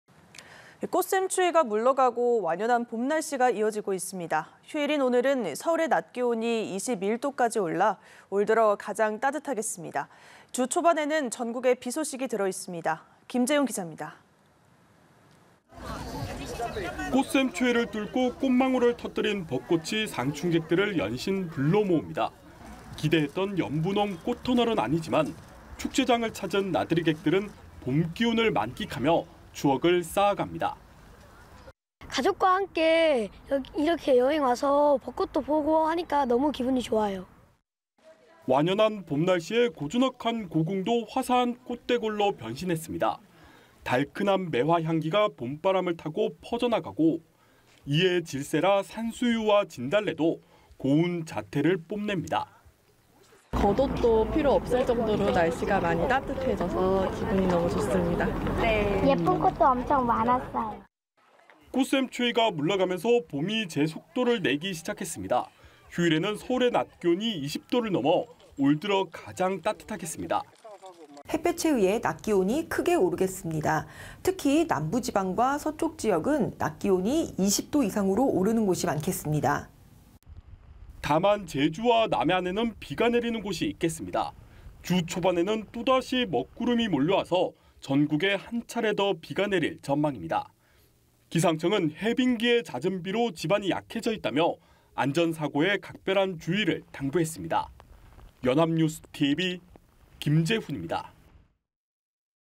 꽃샘추위 뚫고 봄꽃 '활짝'…오늘 올 들어 가장 따뜻 / 연합뉴스TV (YonhapnewsTV)
꽃샘추위 뚫고 봄꽃 '활짝'…오늘 올 들어 가장 따뜻

[앵커]

꽃샘추위가 물러가고 완연한 봄 날씨가 이어지고 있습니다.

휴일인 오늘(24일)은 서울의 낮 기온이 21도까지 올라 올들어 가장 따뜻하겠습니다.

주 초반에는 전국에 비소식이 들어있습니다.

김재훈 기자입니다.

[기자]

꽃샘추위를 뚫고 꽃망울 터뜨린 벚꽃이 상춘객들을 연신 불러 모읍니다.

기대했던 연분홍 꽃터널은 아니지만, 축제장을 찾은 나들이객들은 봄기운을 만끽하며 추억을 쌓아 갑니다.

[장호순·임인애·장라온 / 경기도 가평군] "가족과 함께 이렇게 여행 와서 벚꽃도 보고 하니까 너무 기분이 좋아요"

완연한 봄 날씨에 고즈넉한 고궁도 화사한 꽃대궐로 변신했습니다.

달큰한 매화 향기가 봄바람을 타고 퍼져나가고, 이에 질세라 산수유와 진달래도 고운 자태를 뽐냅니다.

[이상미 / 서울시 마포구] "겉옷도 필요 없을 정도로 날씨가 따뜻해서 기분이 너무 좋습니다. 예쁜 꽃도 엄청 많았어요."

꽃샘추위가 물러가면서 봄이 제 속도를 내기 시작했습니다.

휴일에는 서울의 낮 기온이 20도를 넘어 올 들어 가장 따뜻하겠습니다.

[노유진 / 기상청 예보분석관] "햇볕에 의해 낮 기온이 크게 오르겠습니다. 특히, 남부지방과 서쪽지역은 낮 기온이 20도 이상으로 오르는 곳이 많겠습니다."

다만 제주와 남해안에는 비가 내리는 곳이 있겠습니다.

주 초반에는 또다시 먹구름이 몰려와서 전국에 한차례 더 비가 내릴 전망입니다.

기상청은 해빙기에 잦은 비로 지반이 약해져 있다며, 안전사고에 각별한 주의를 당부했습니다.

[영상취재기자 권혁준, 김완기, 이덕훈, 황종호]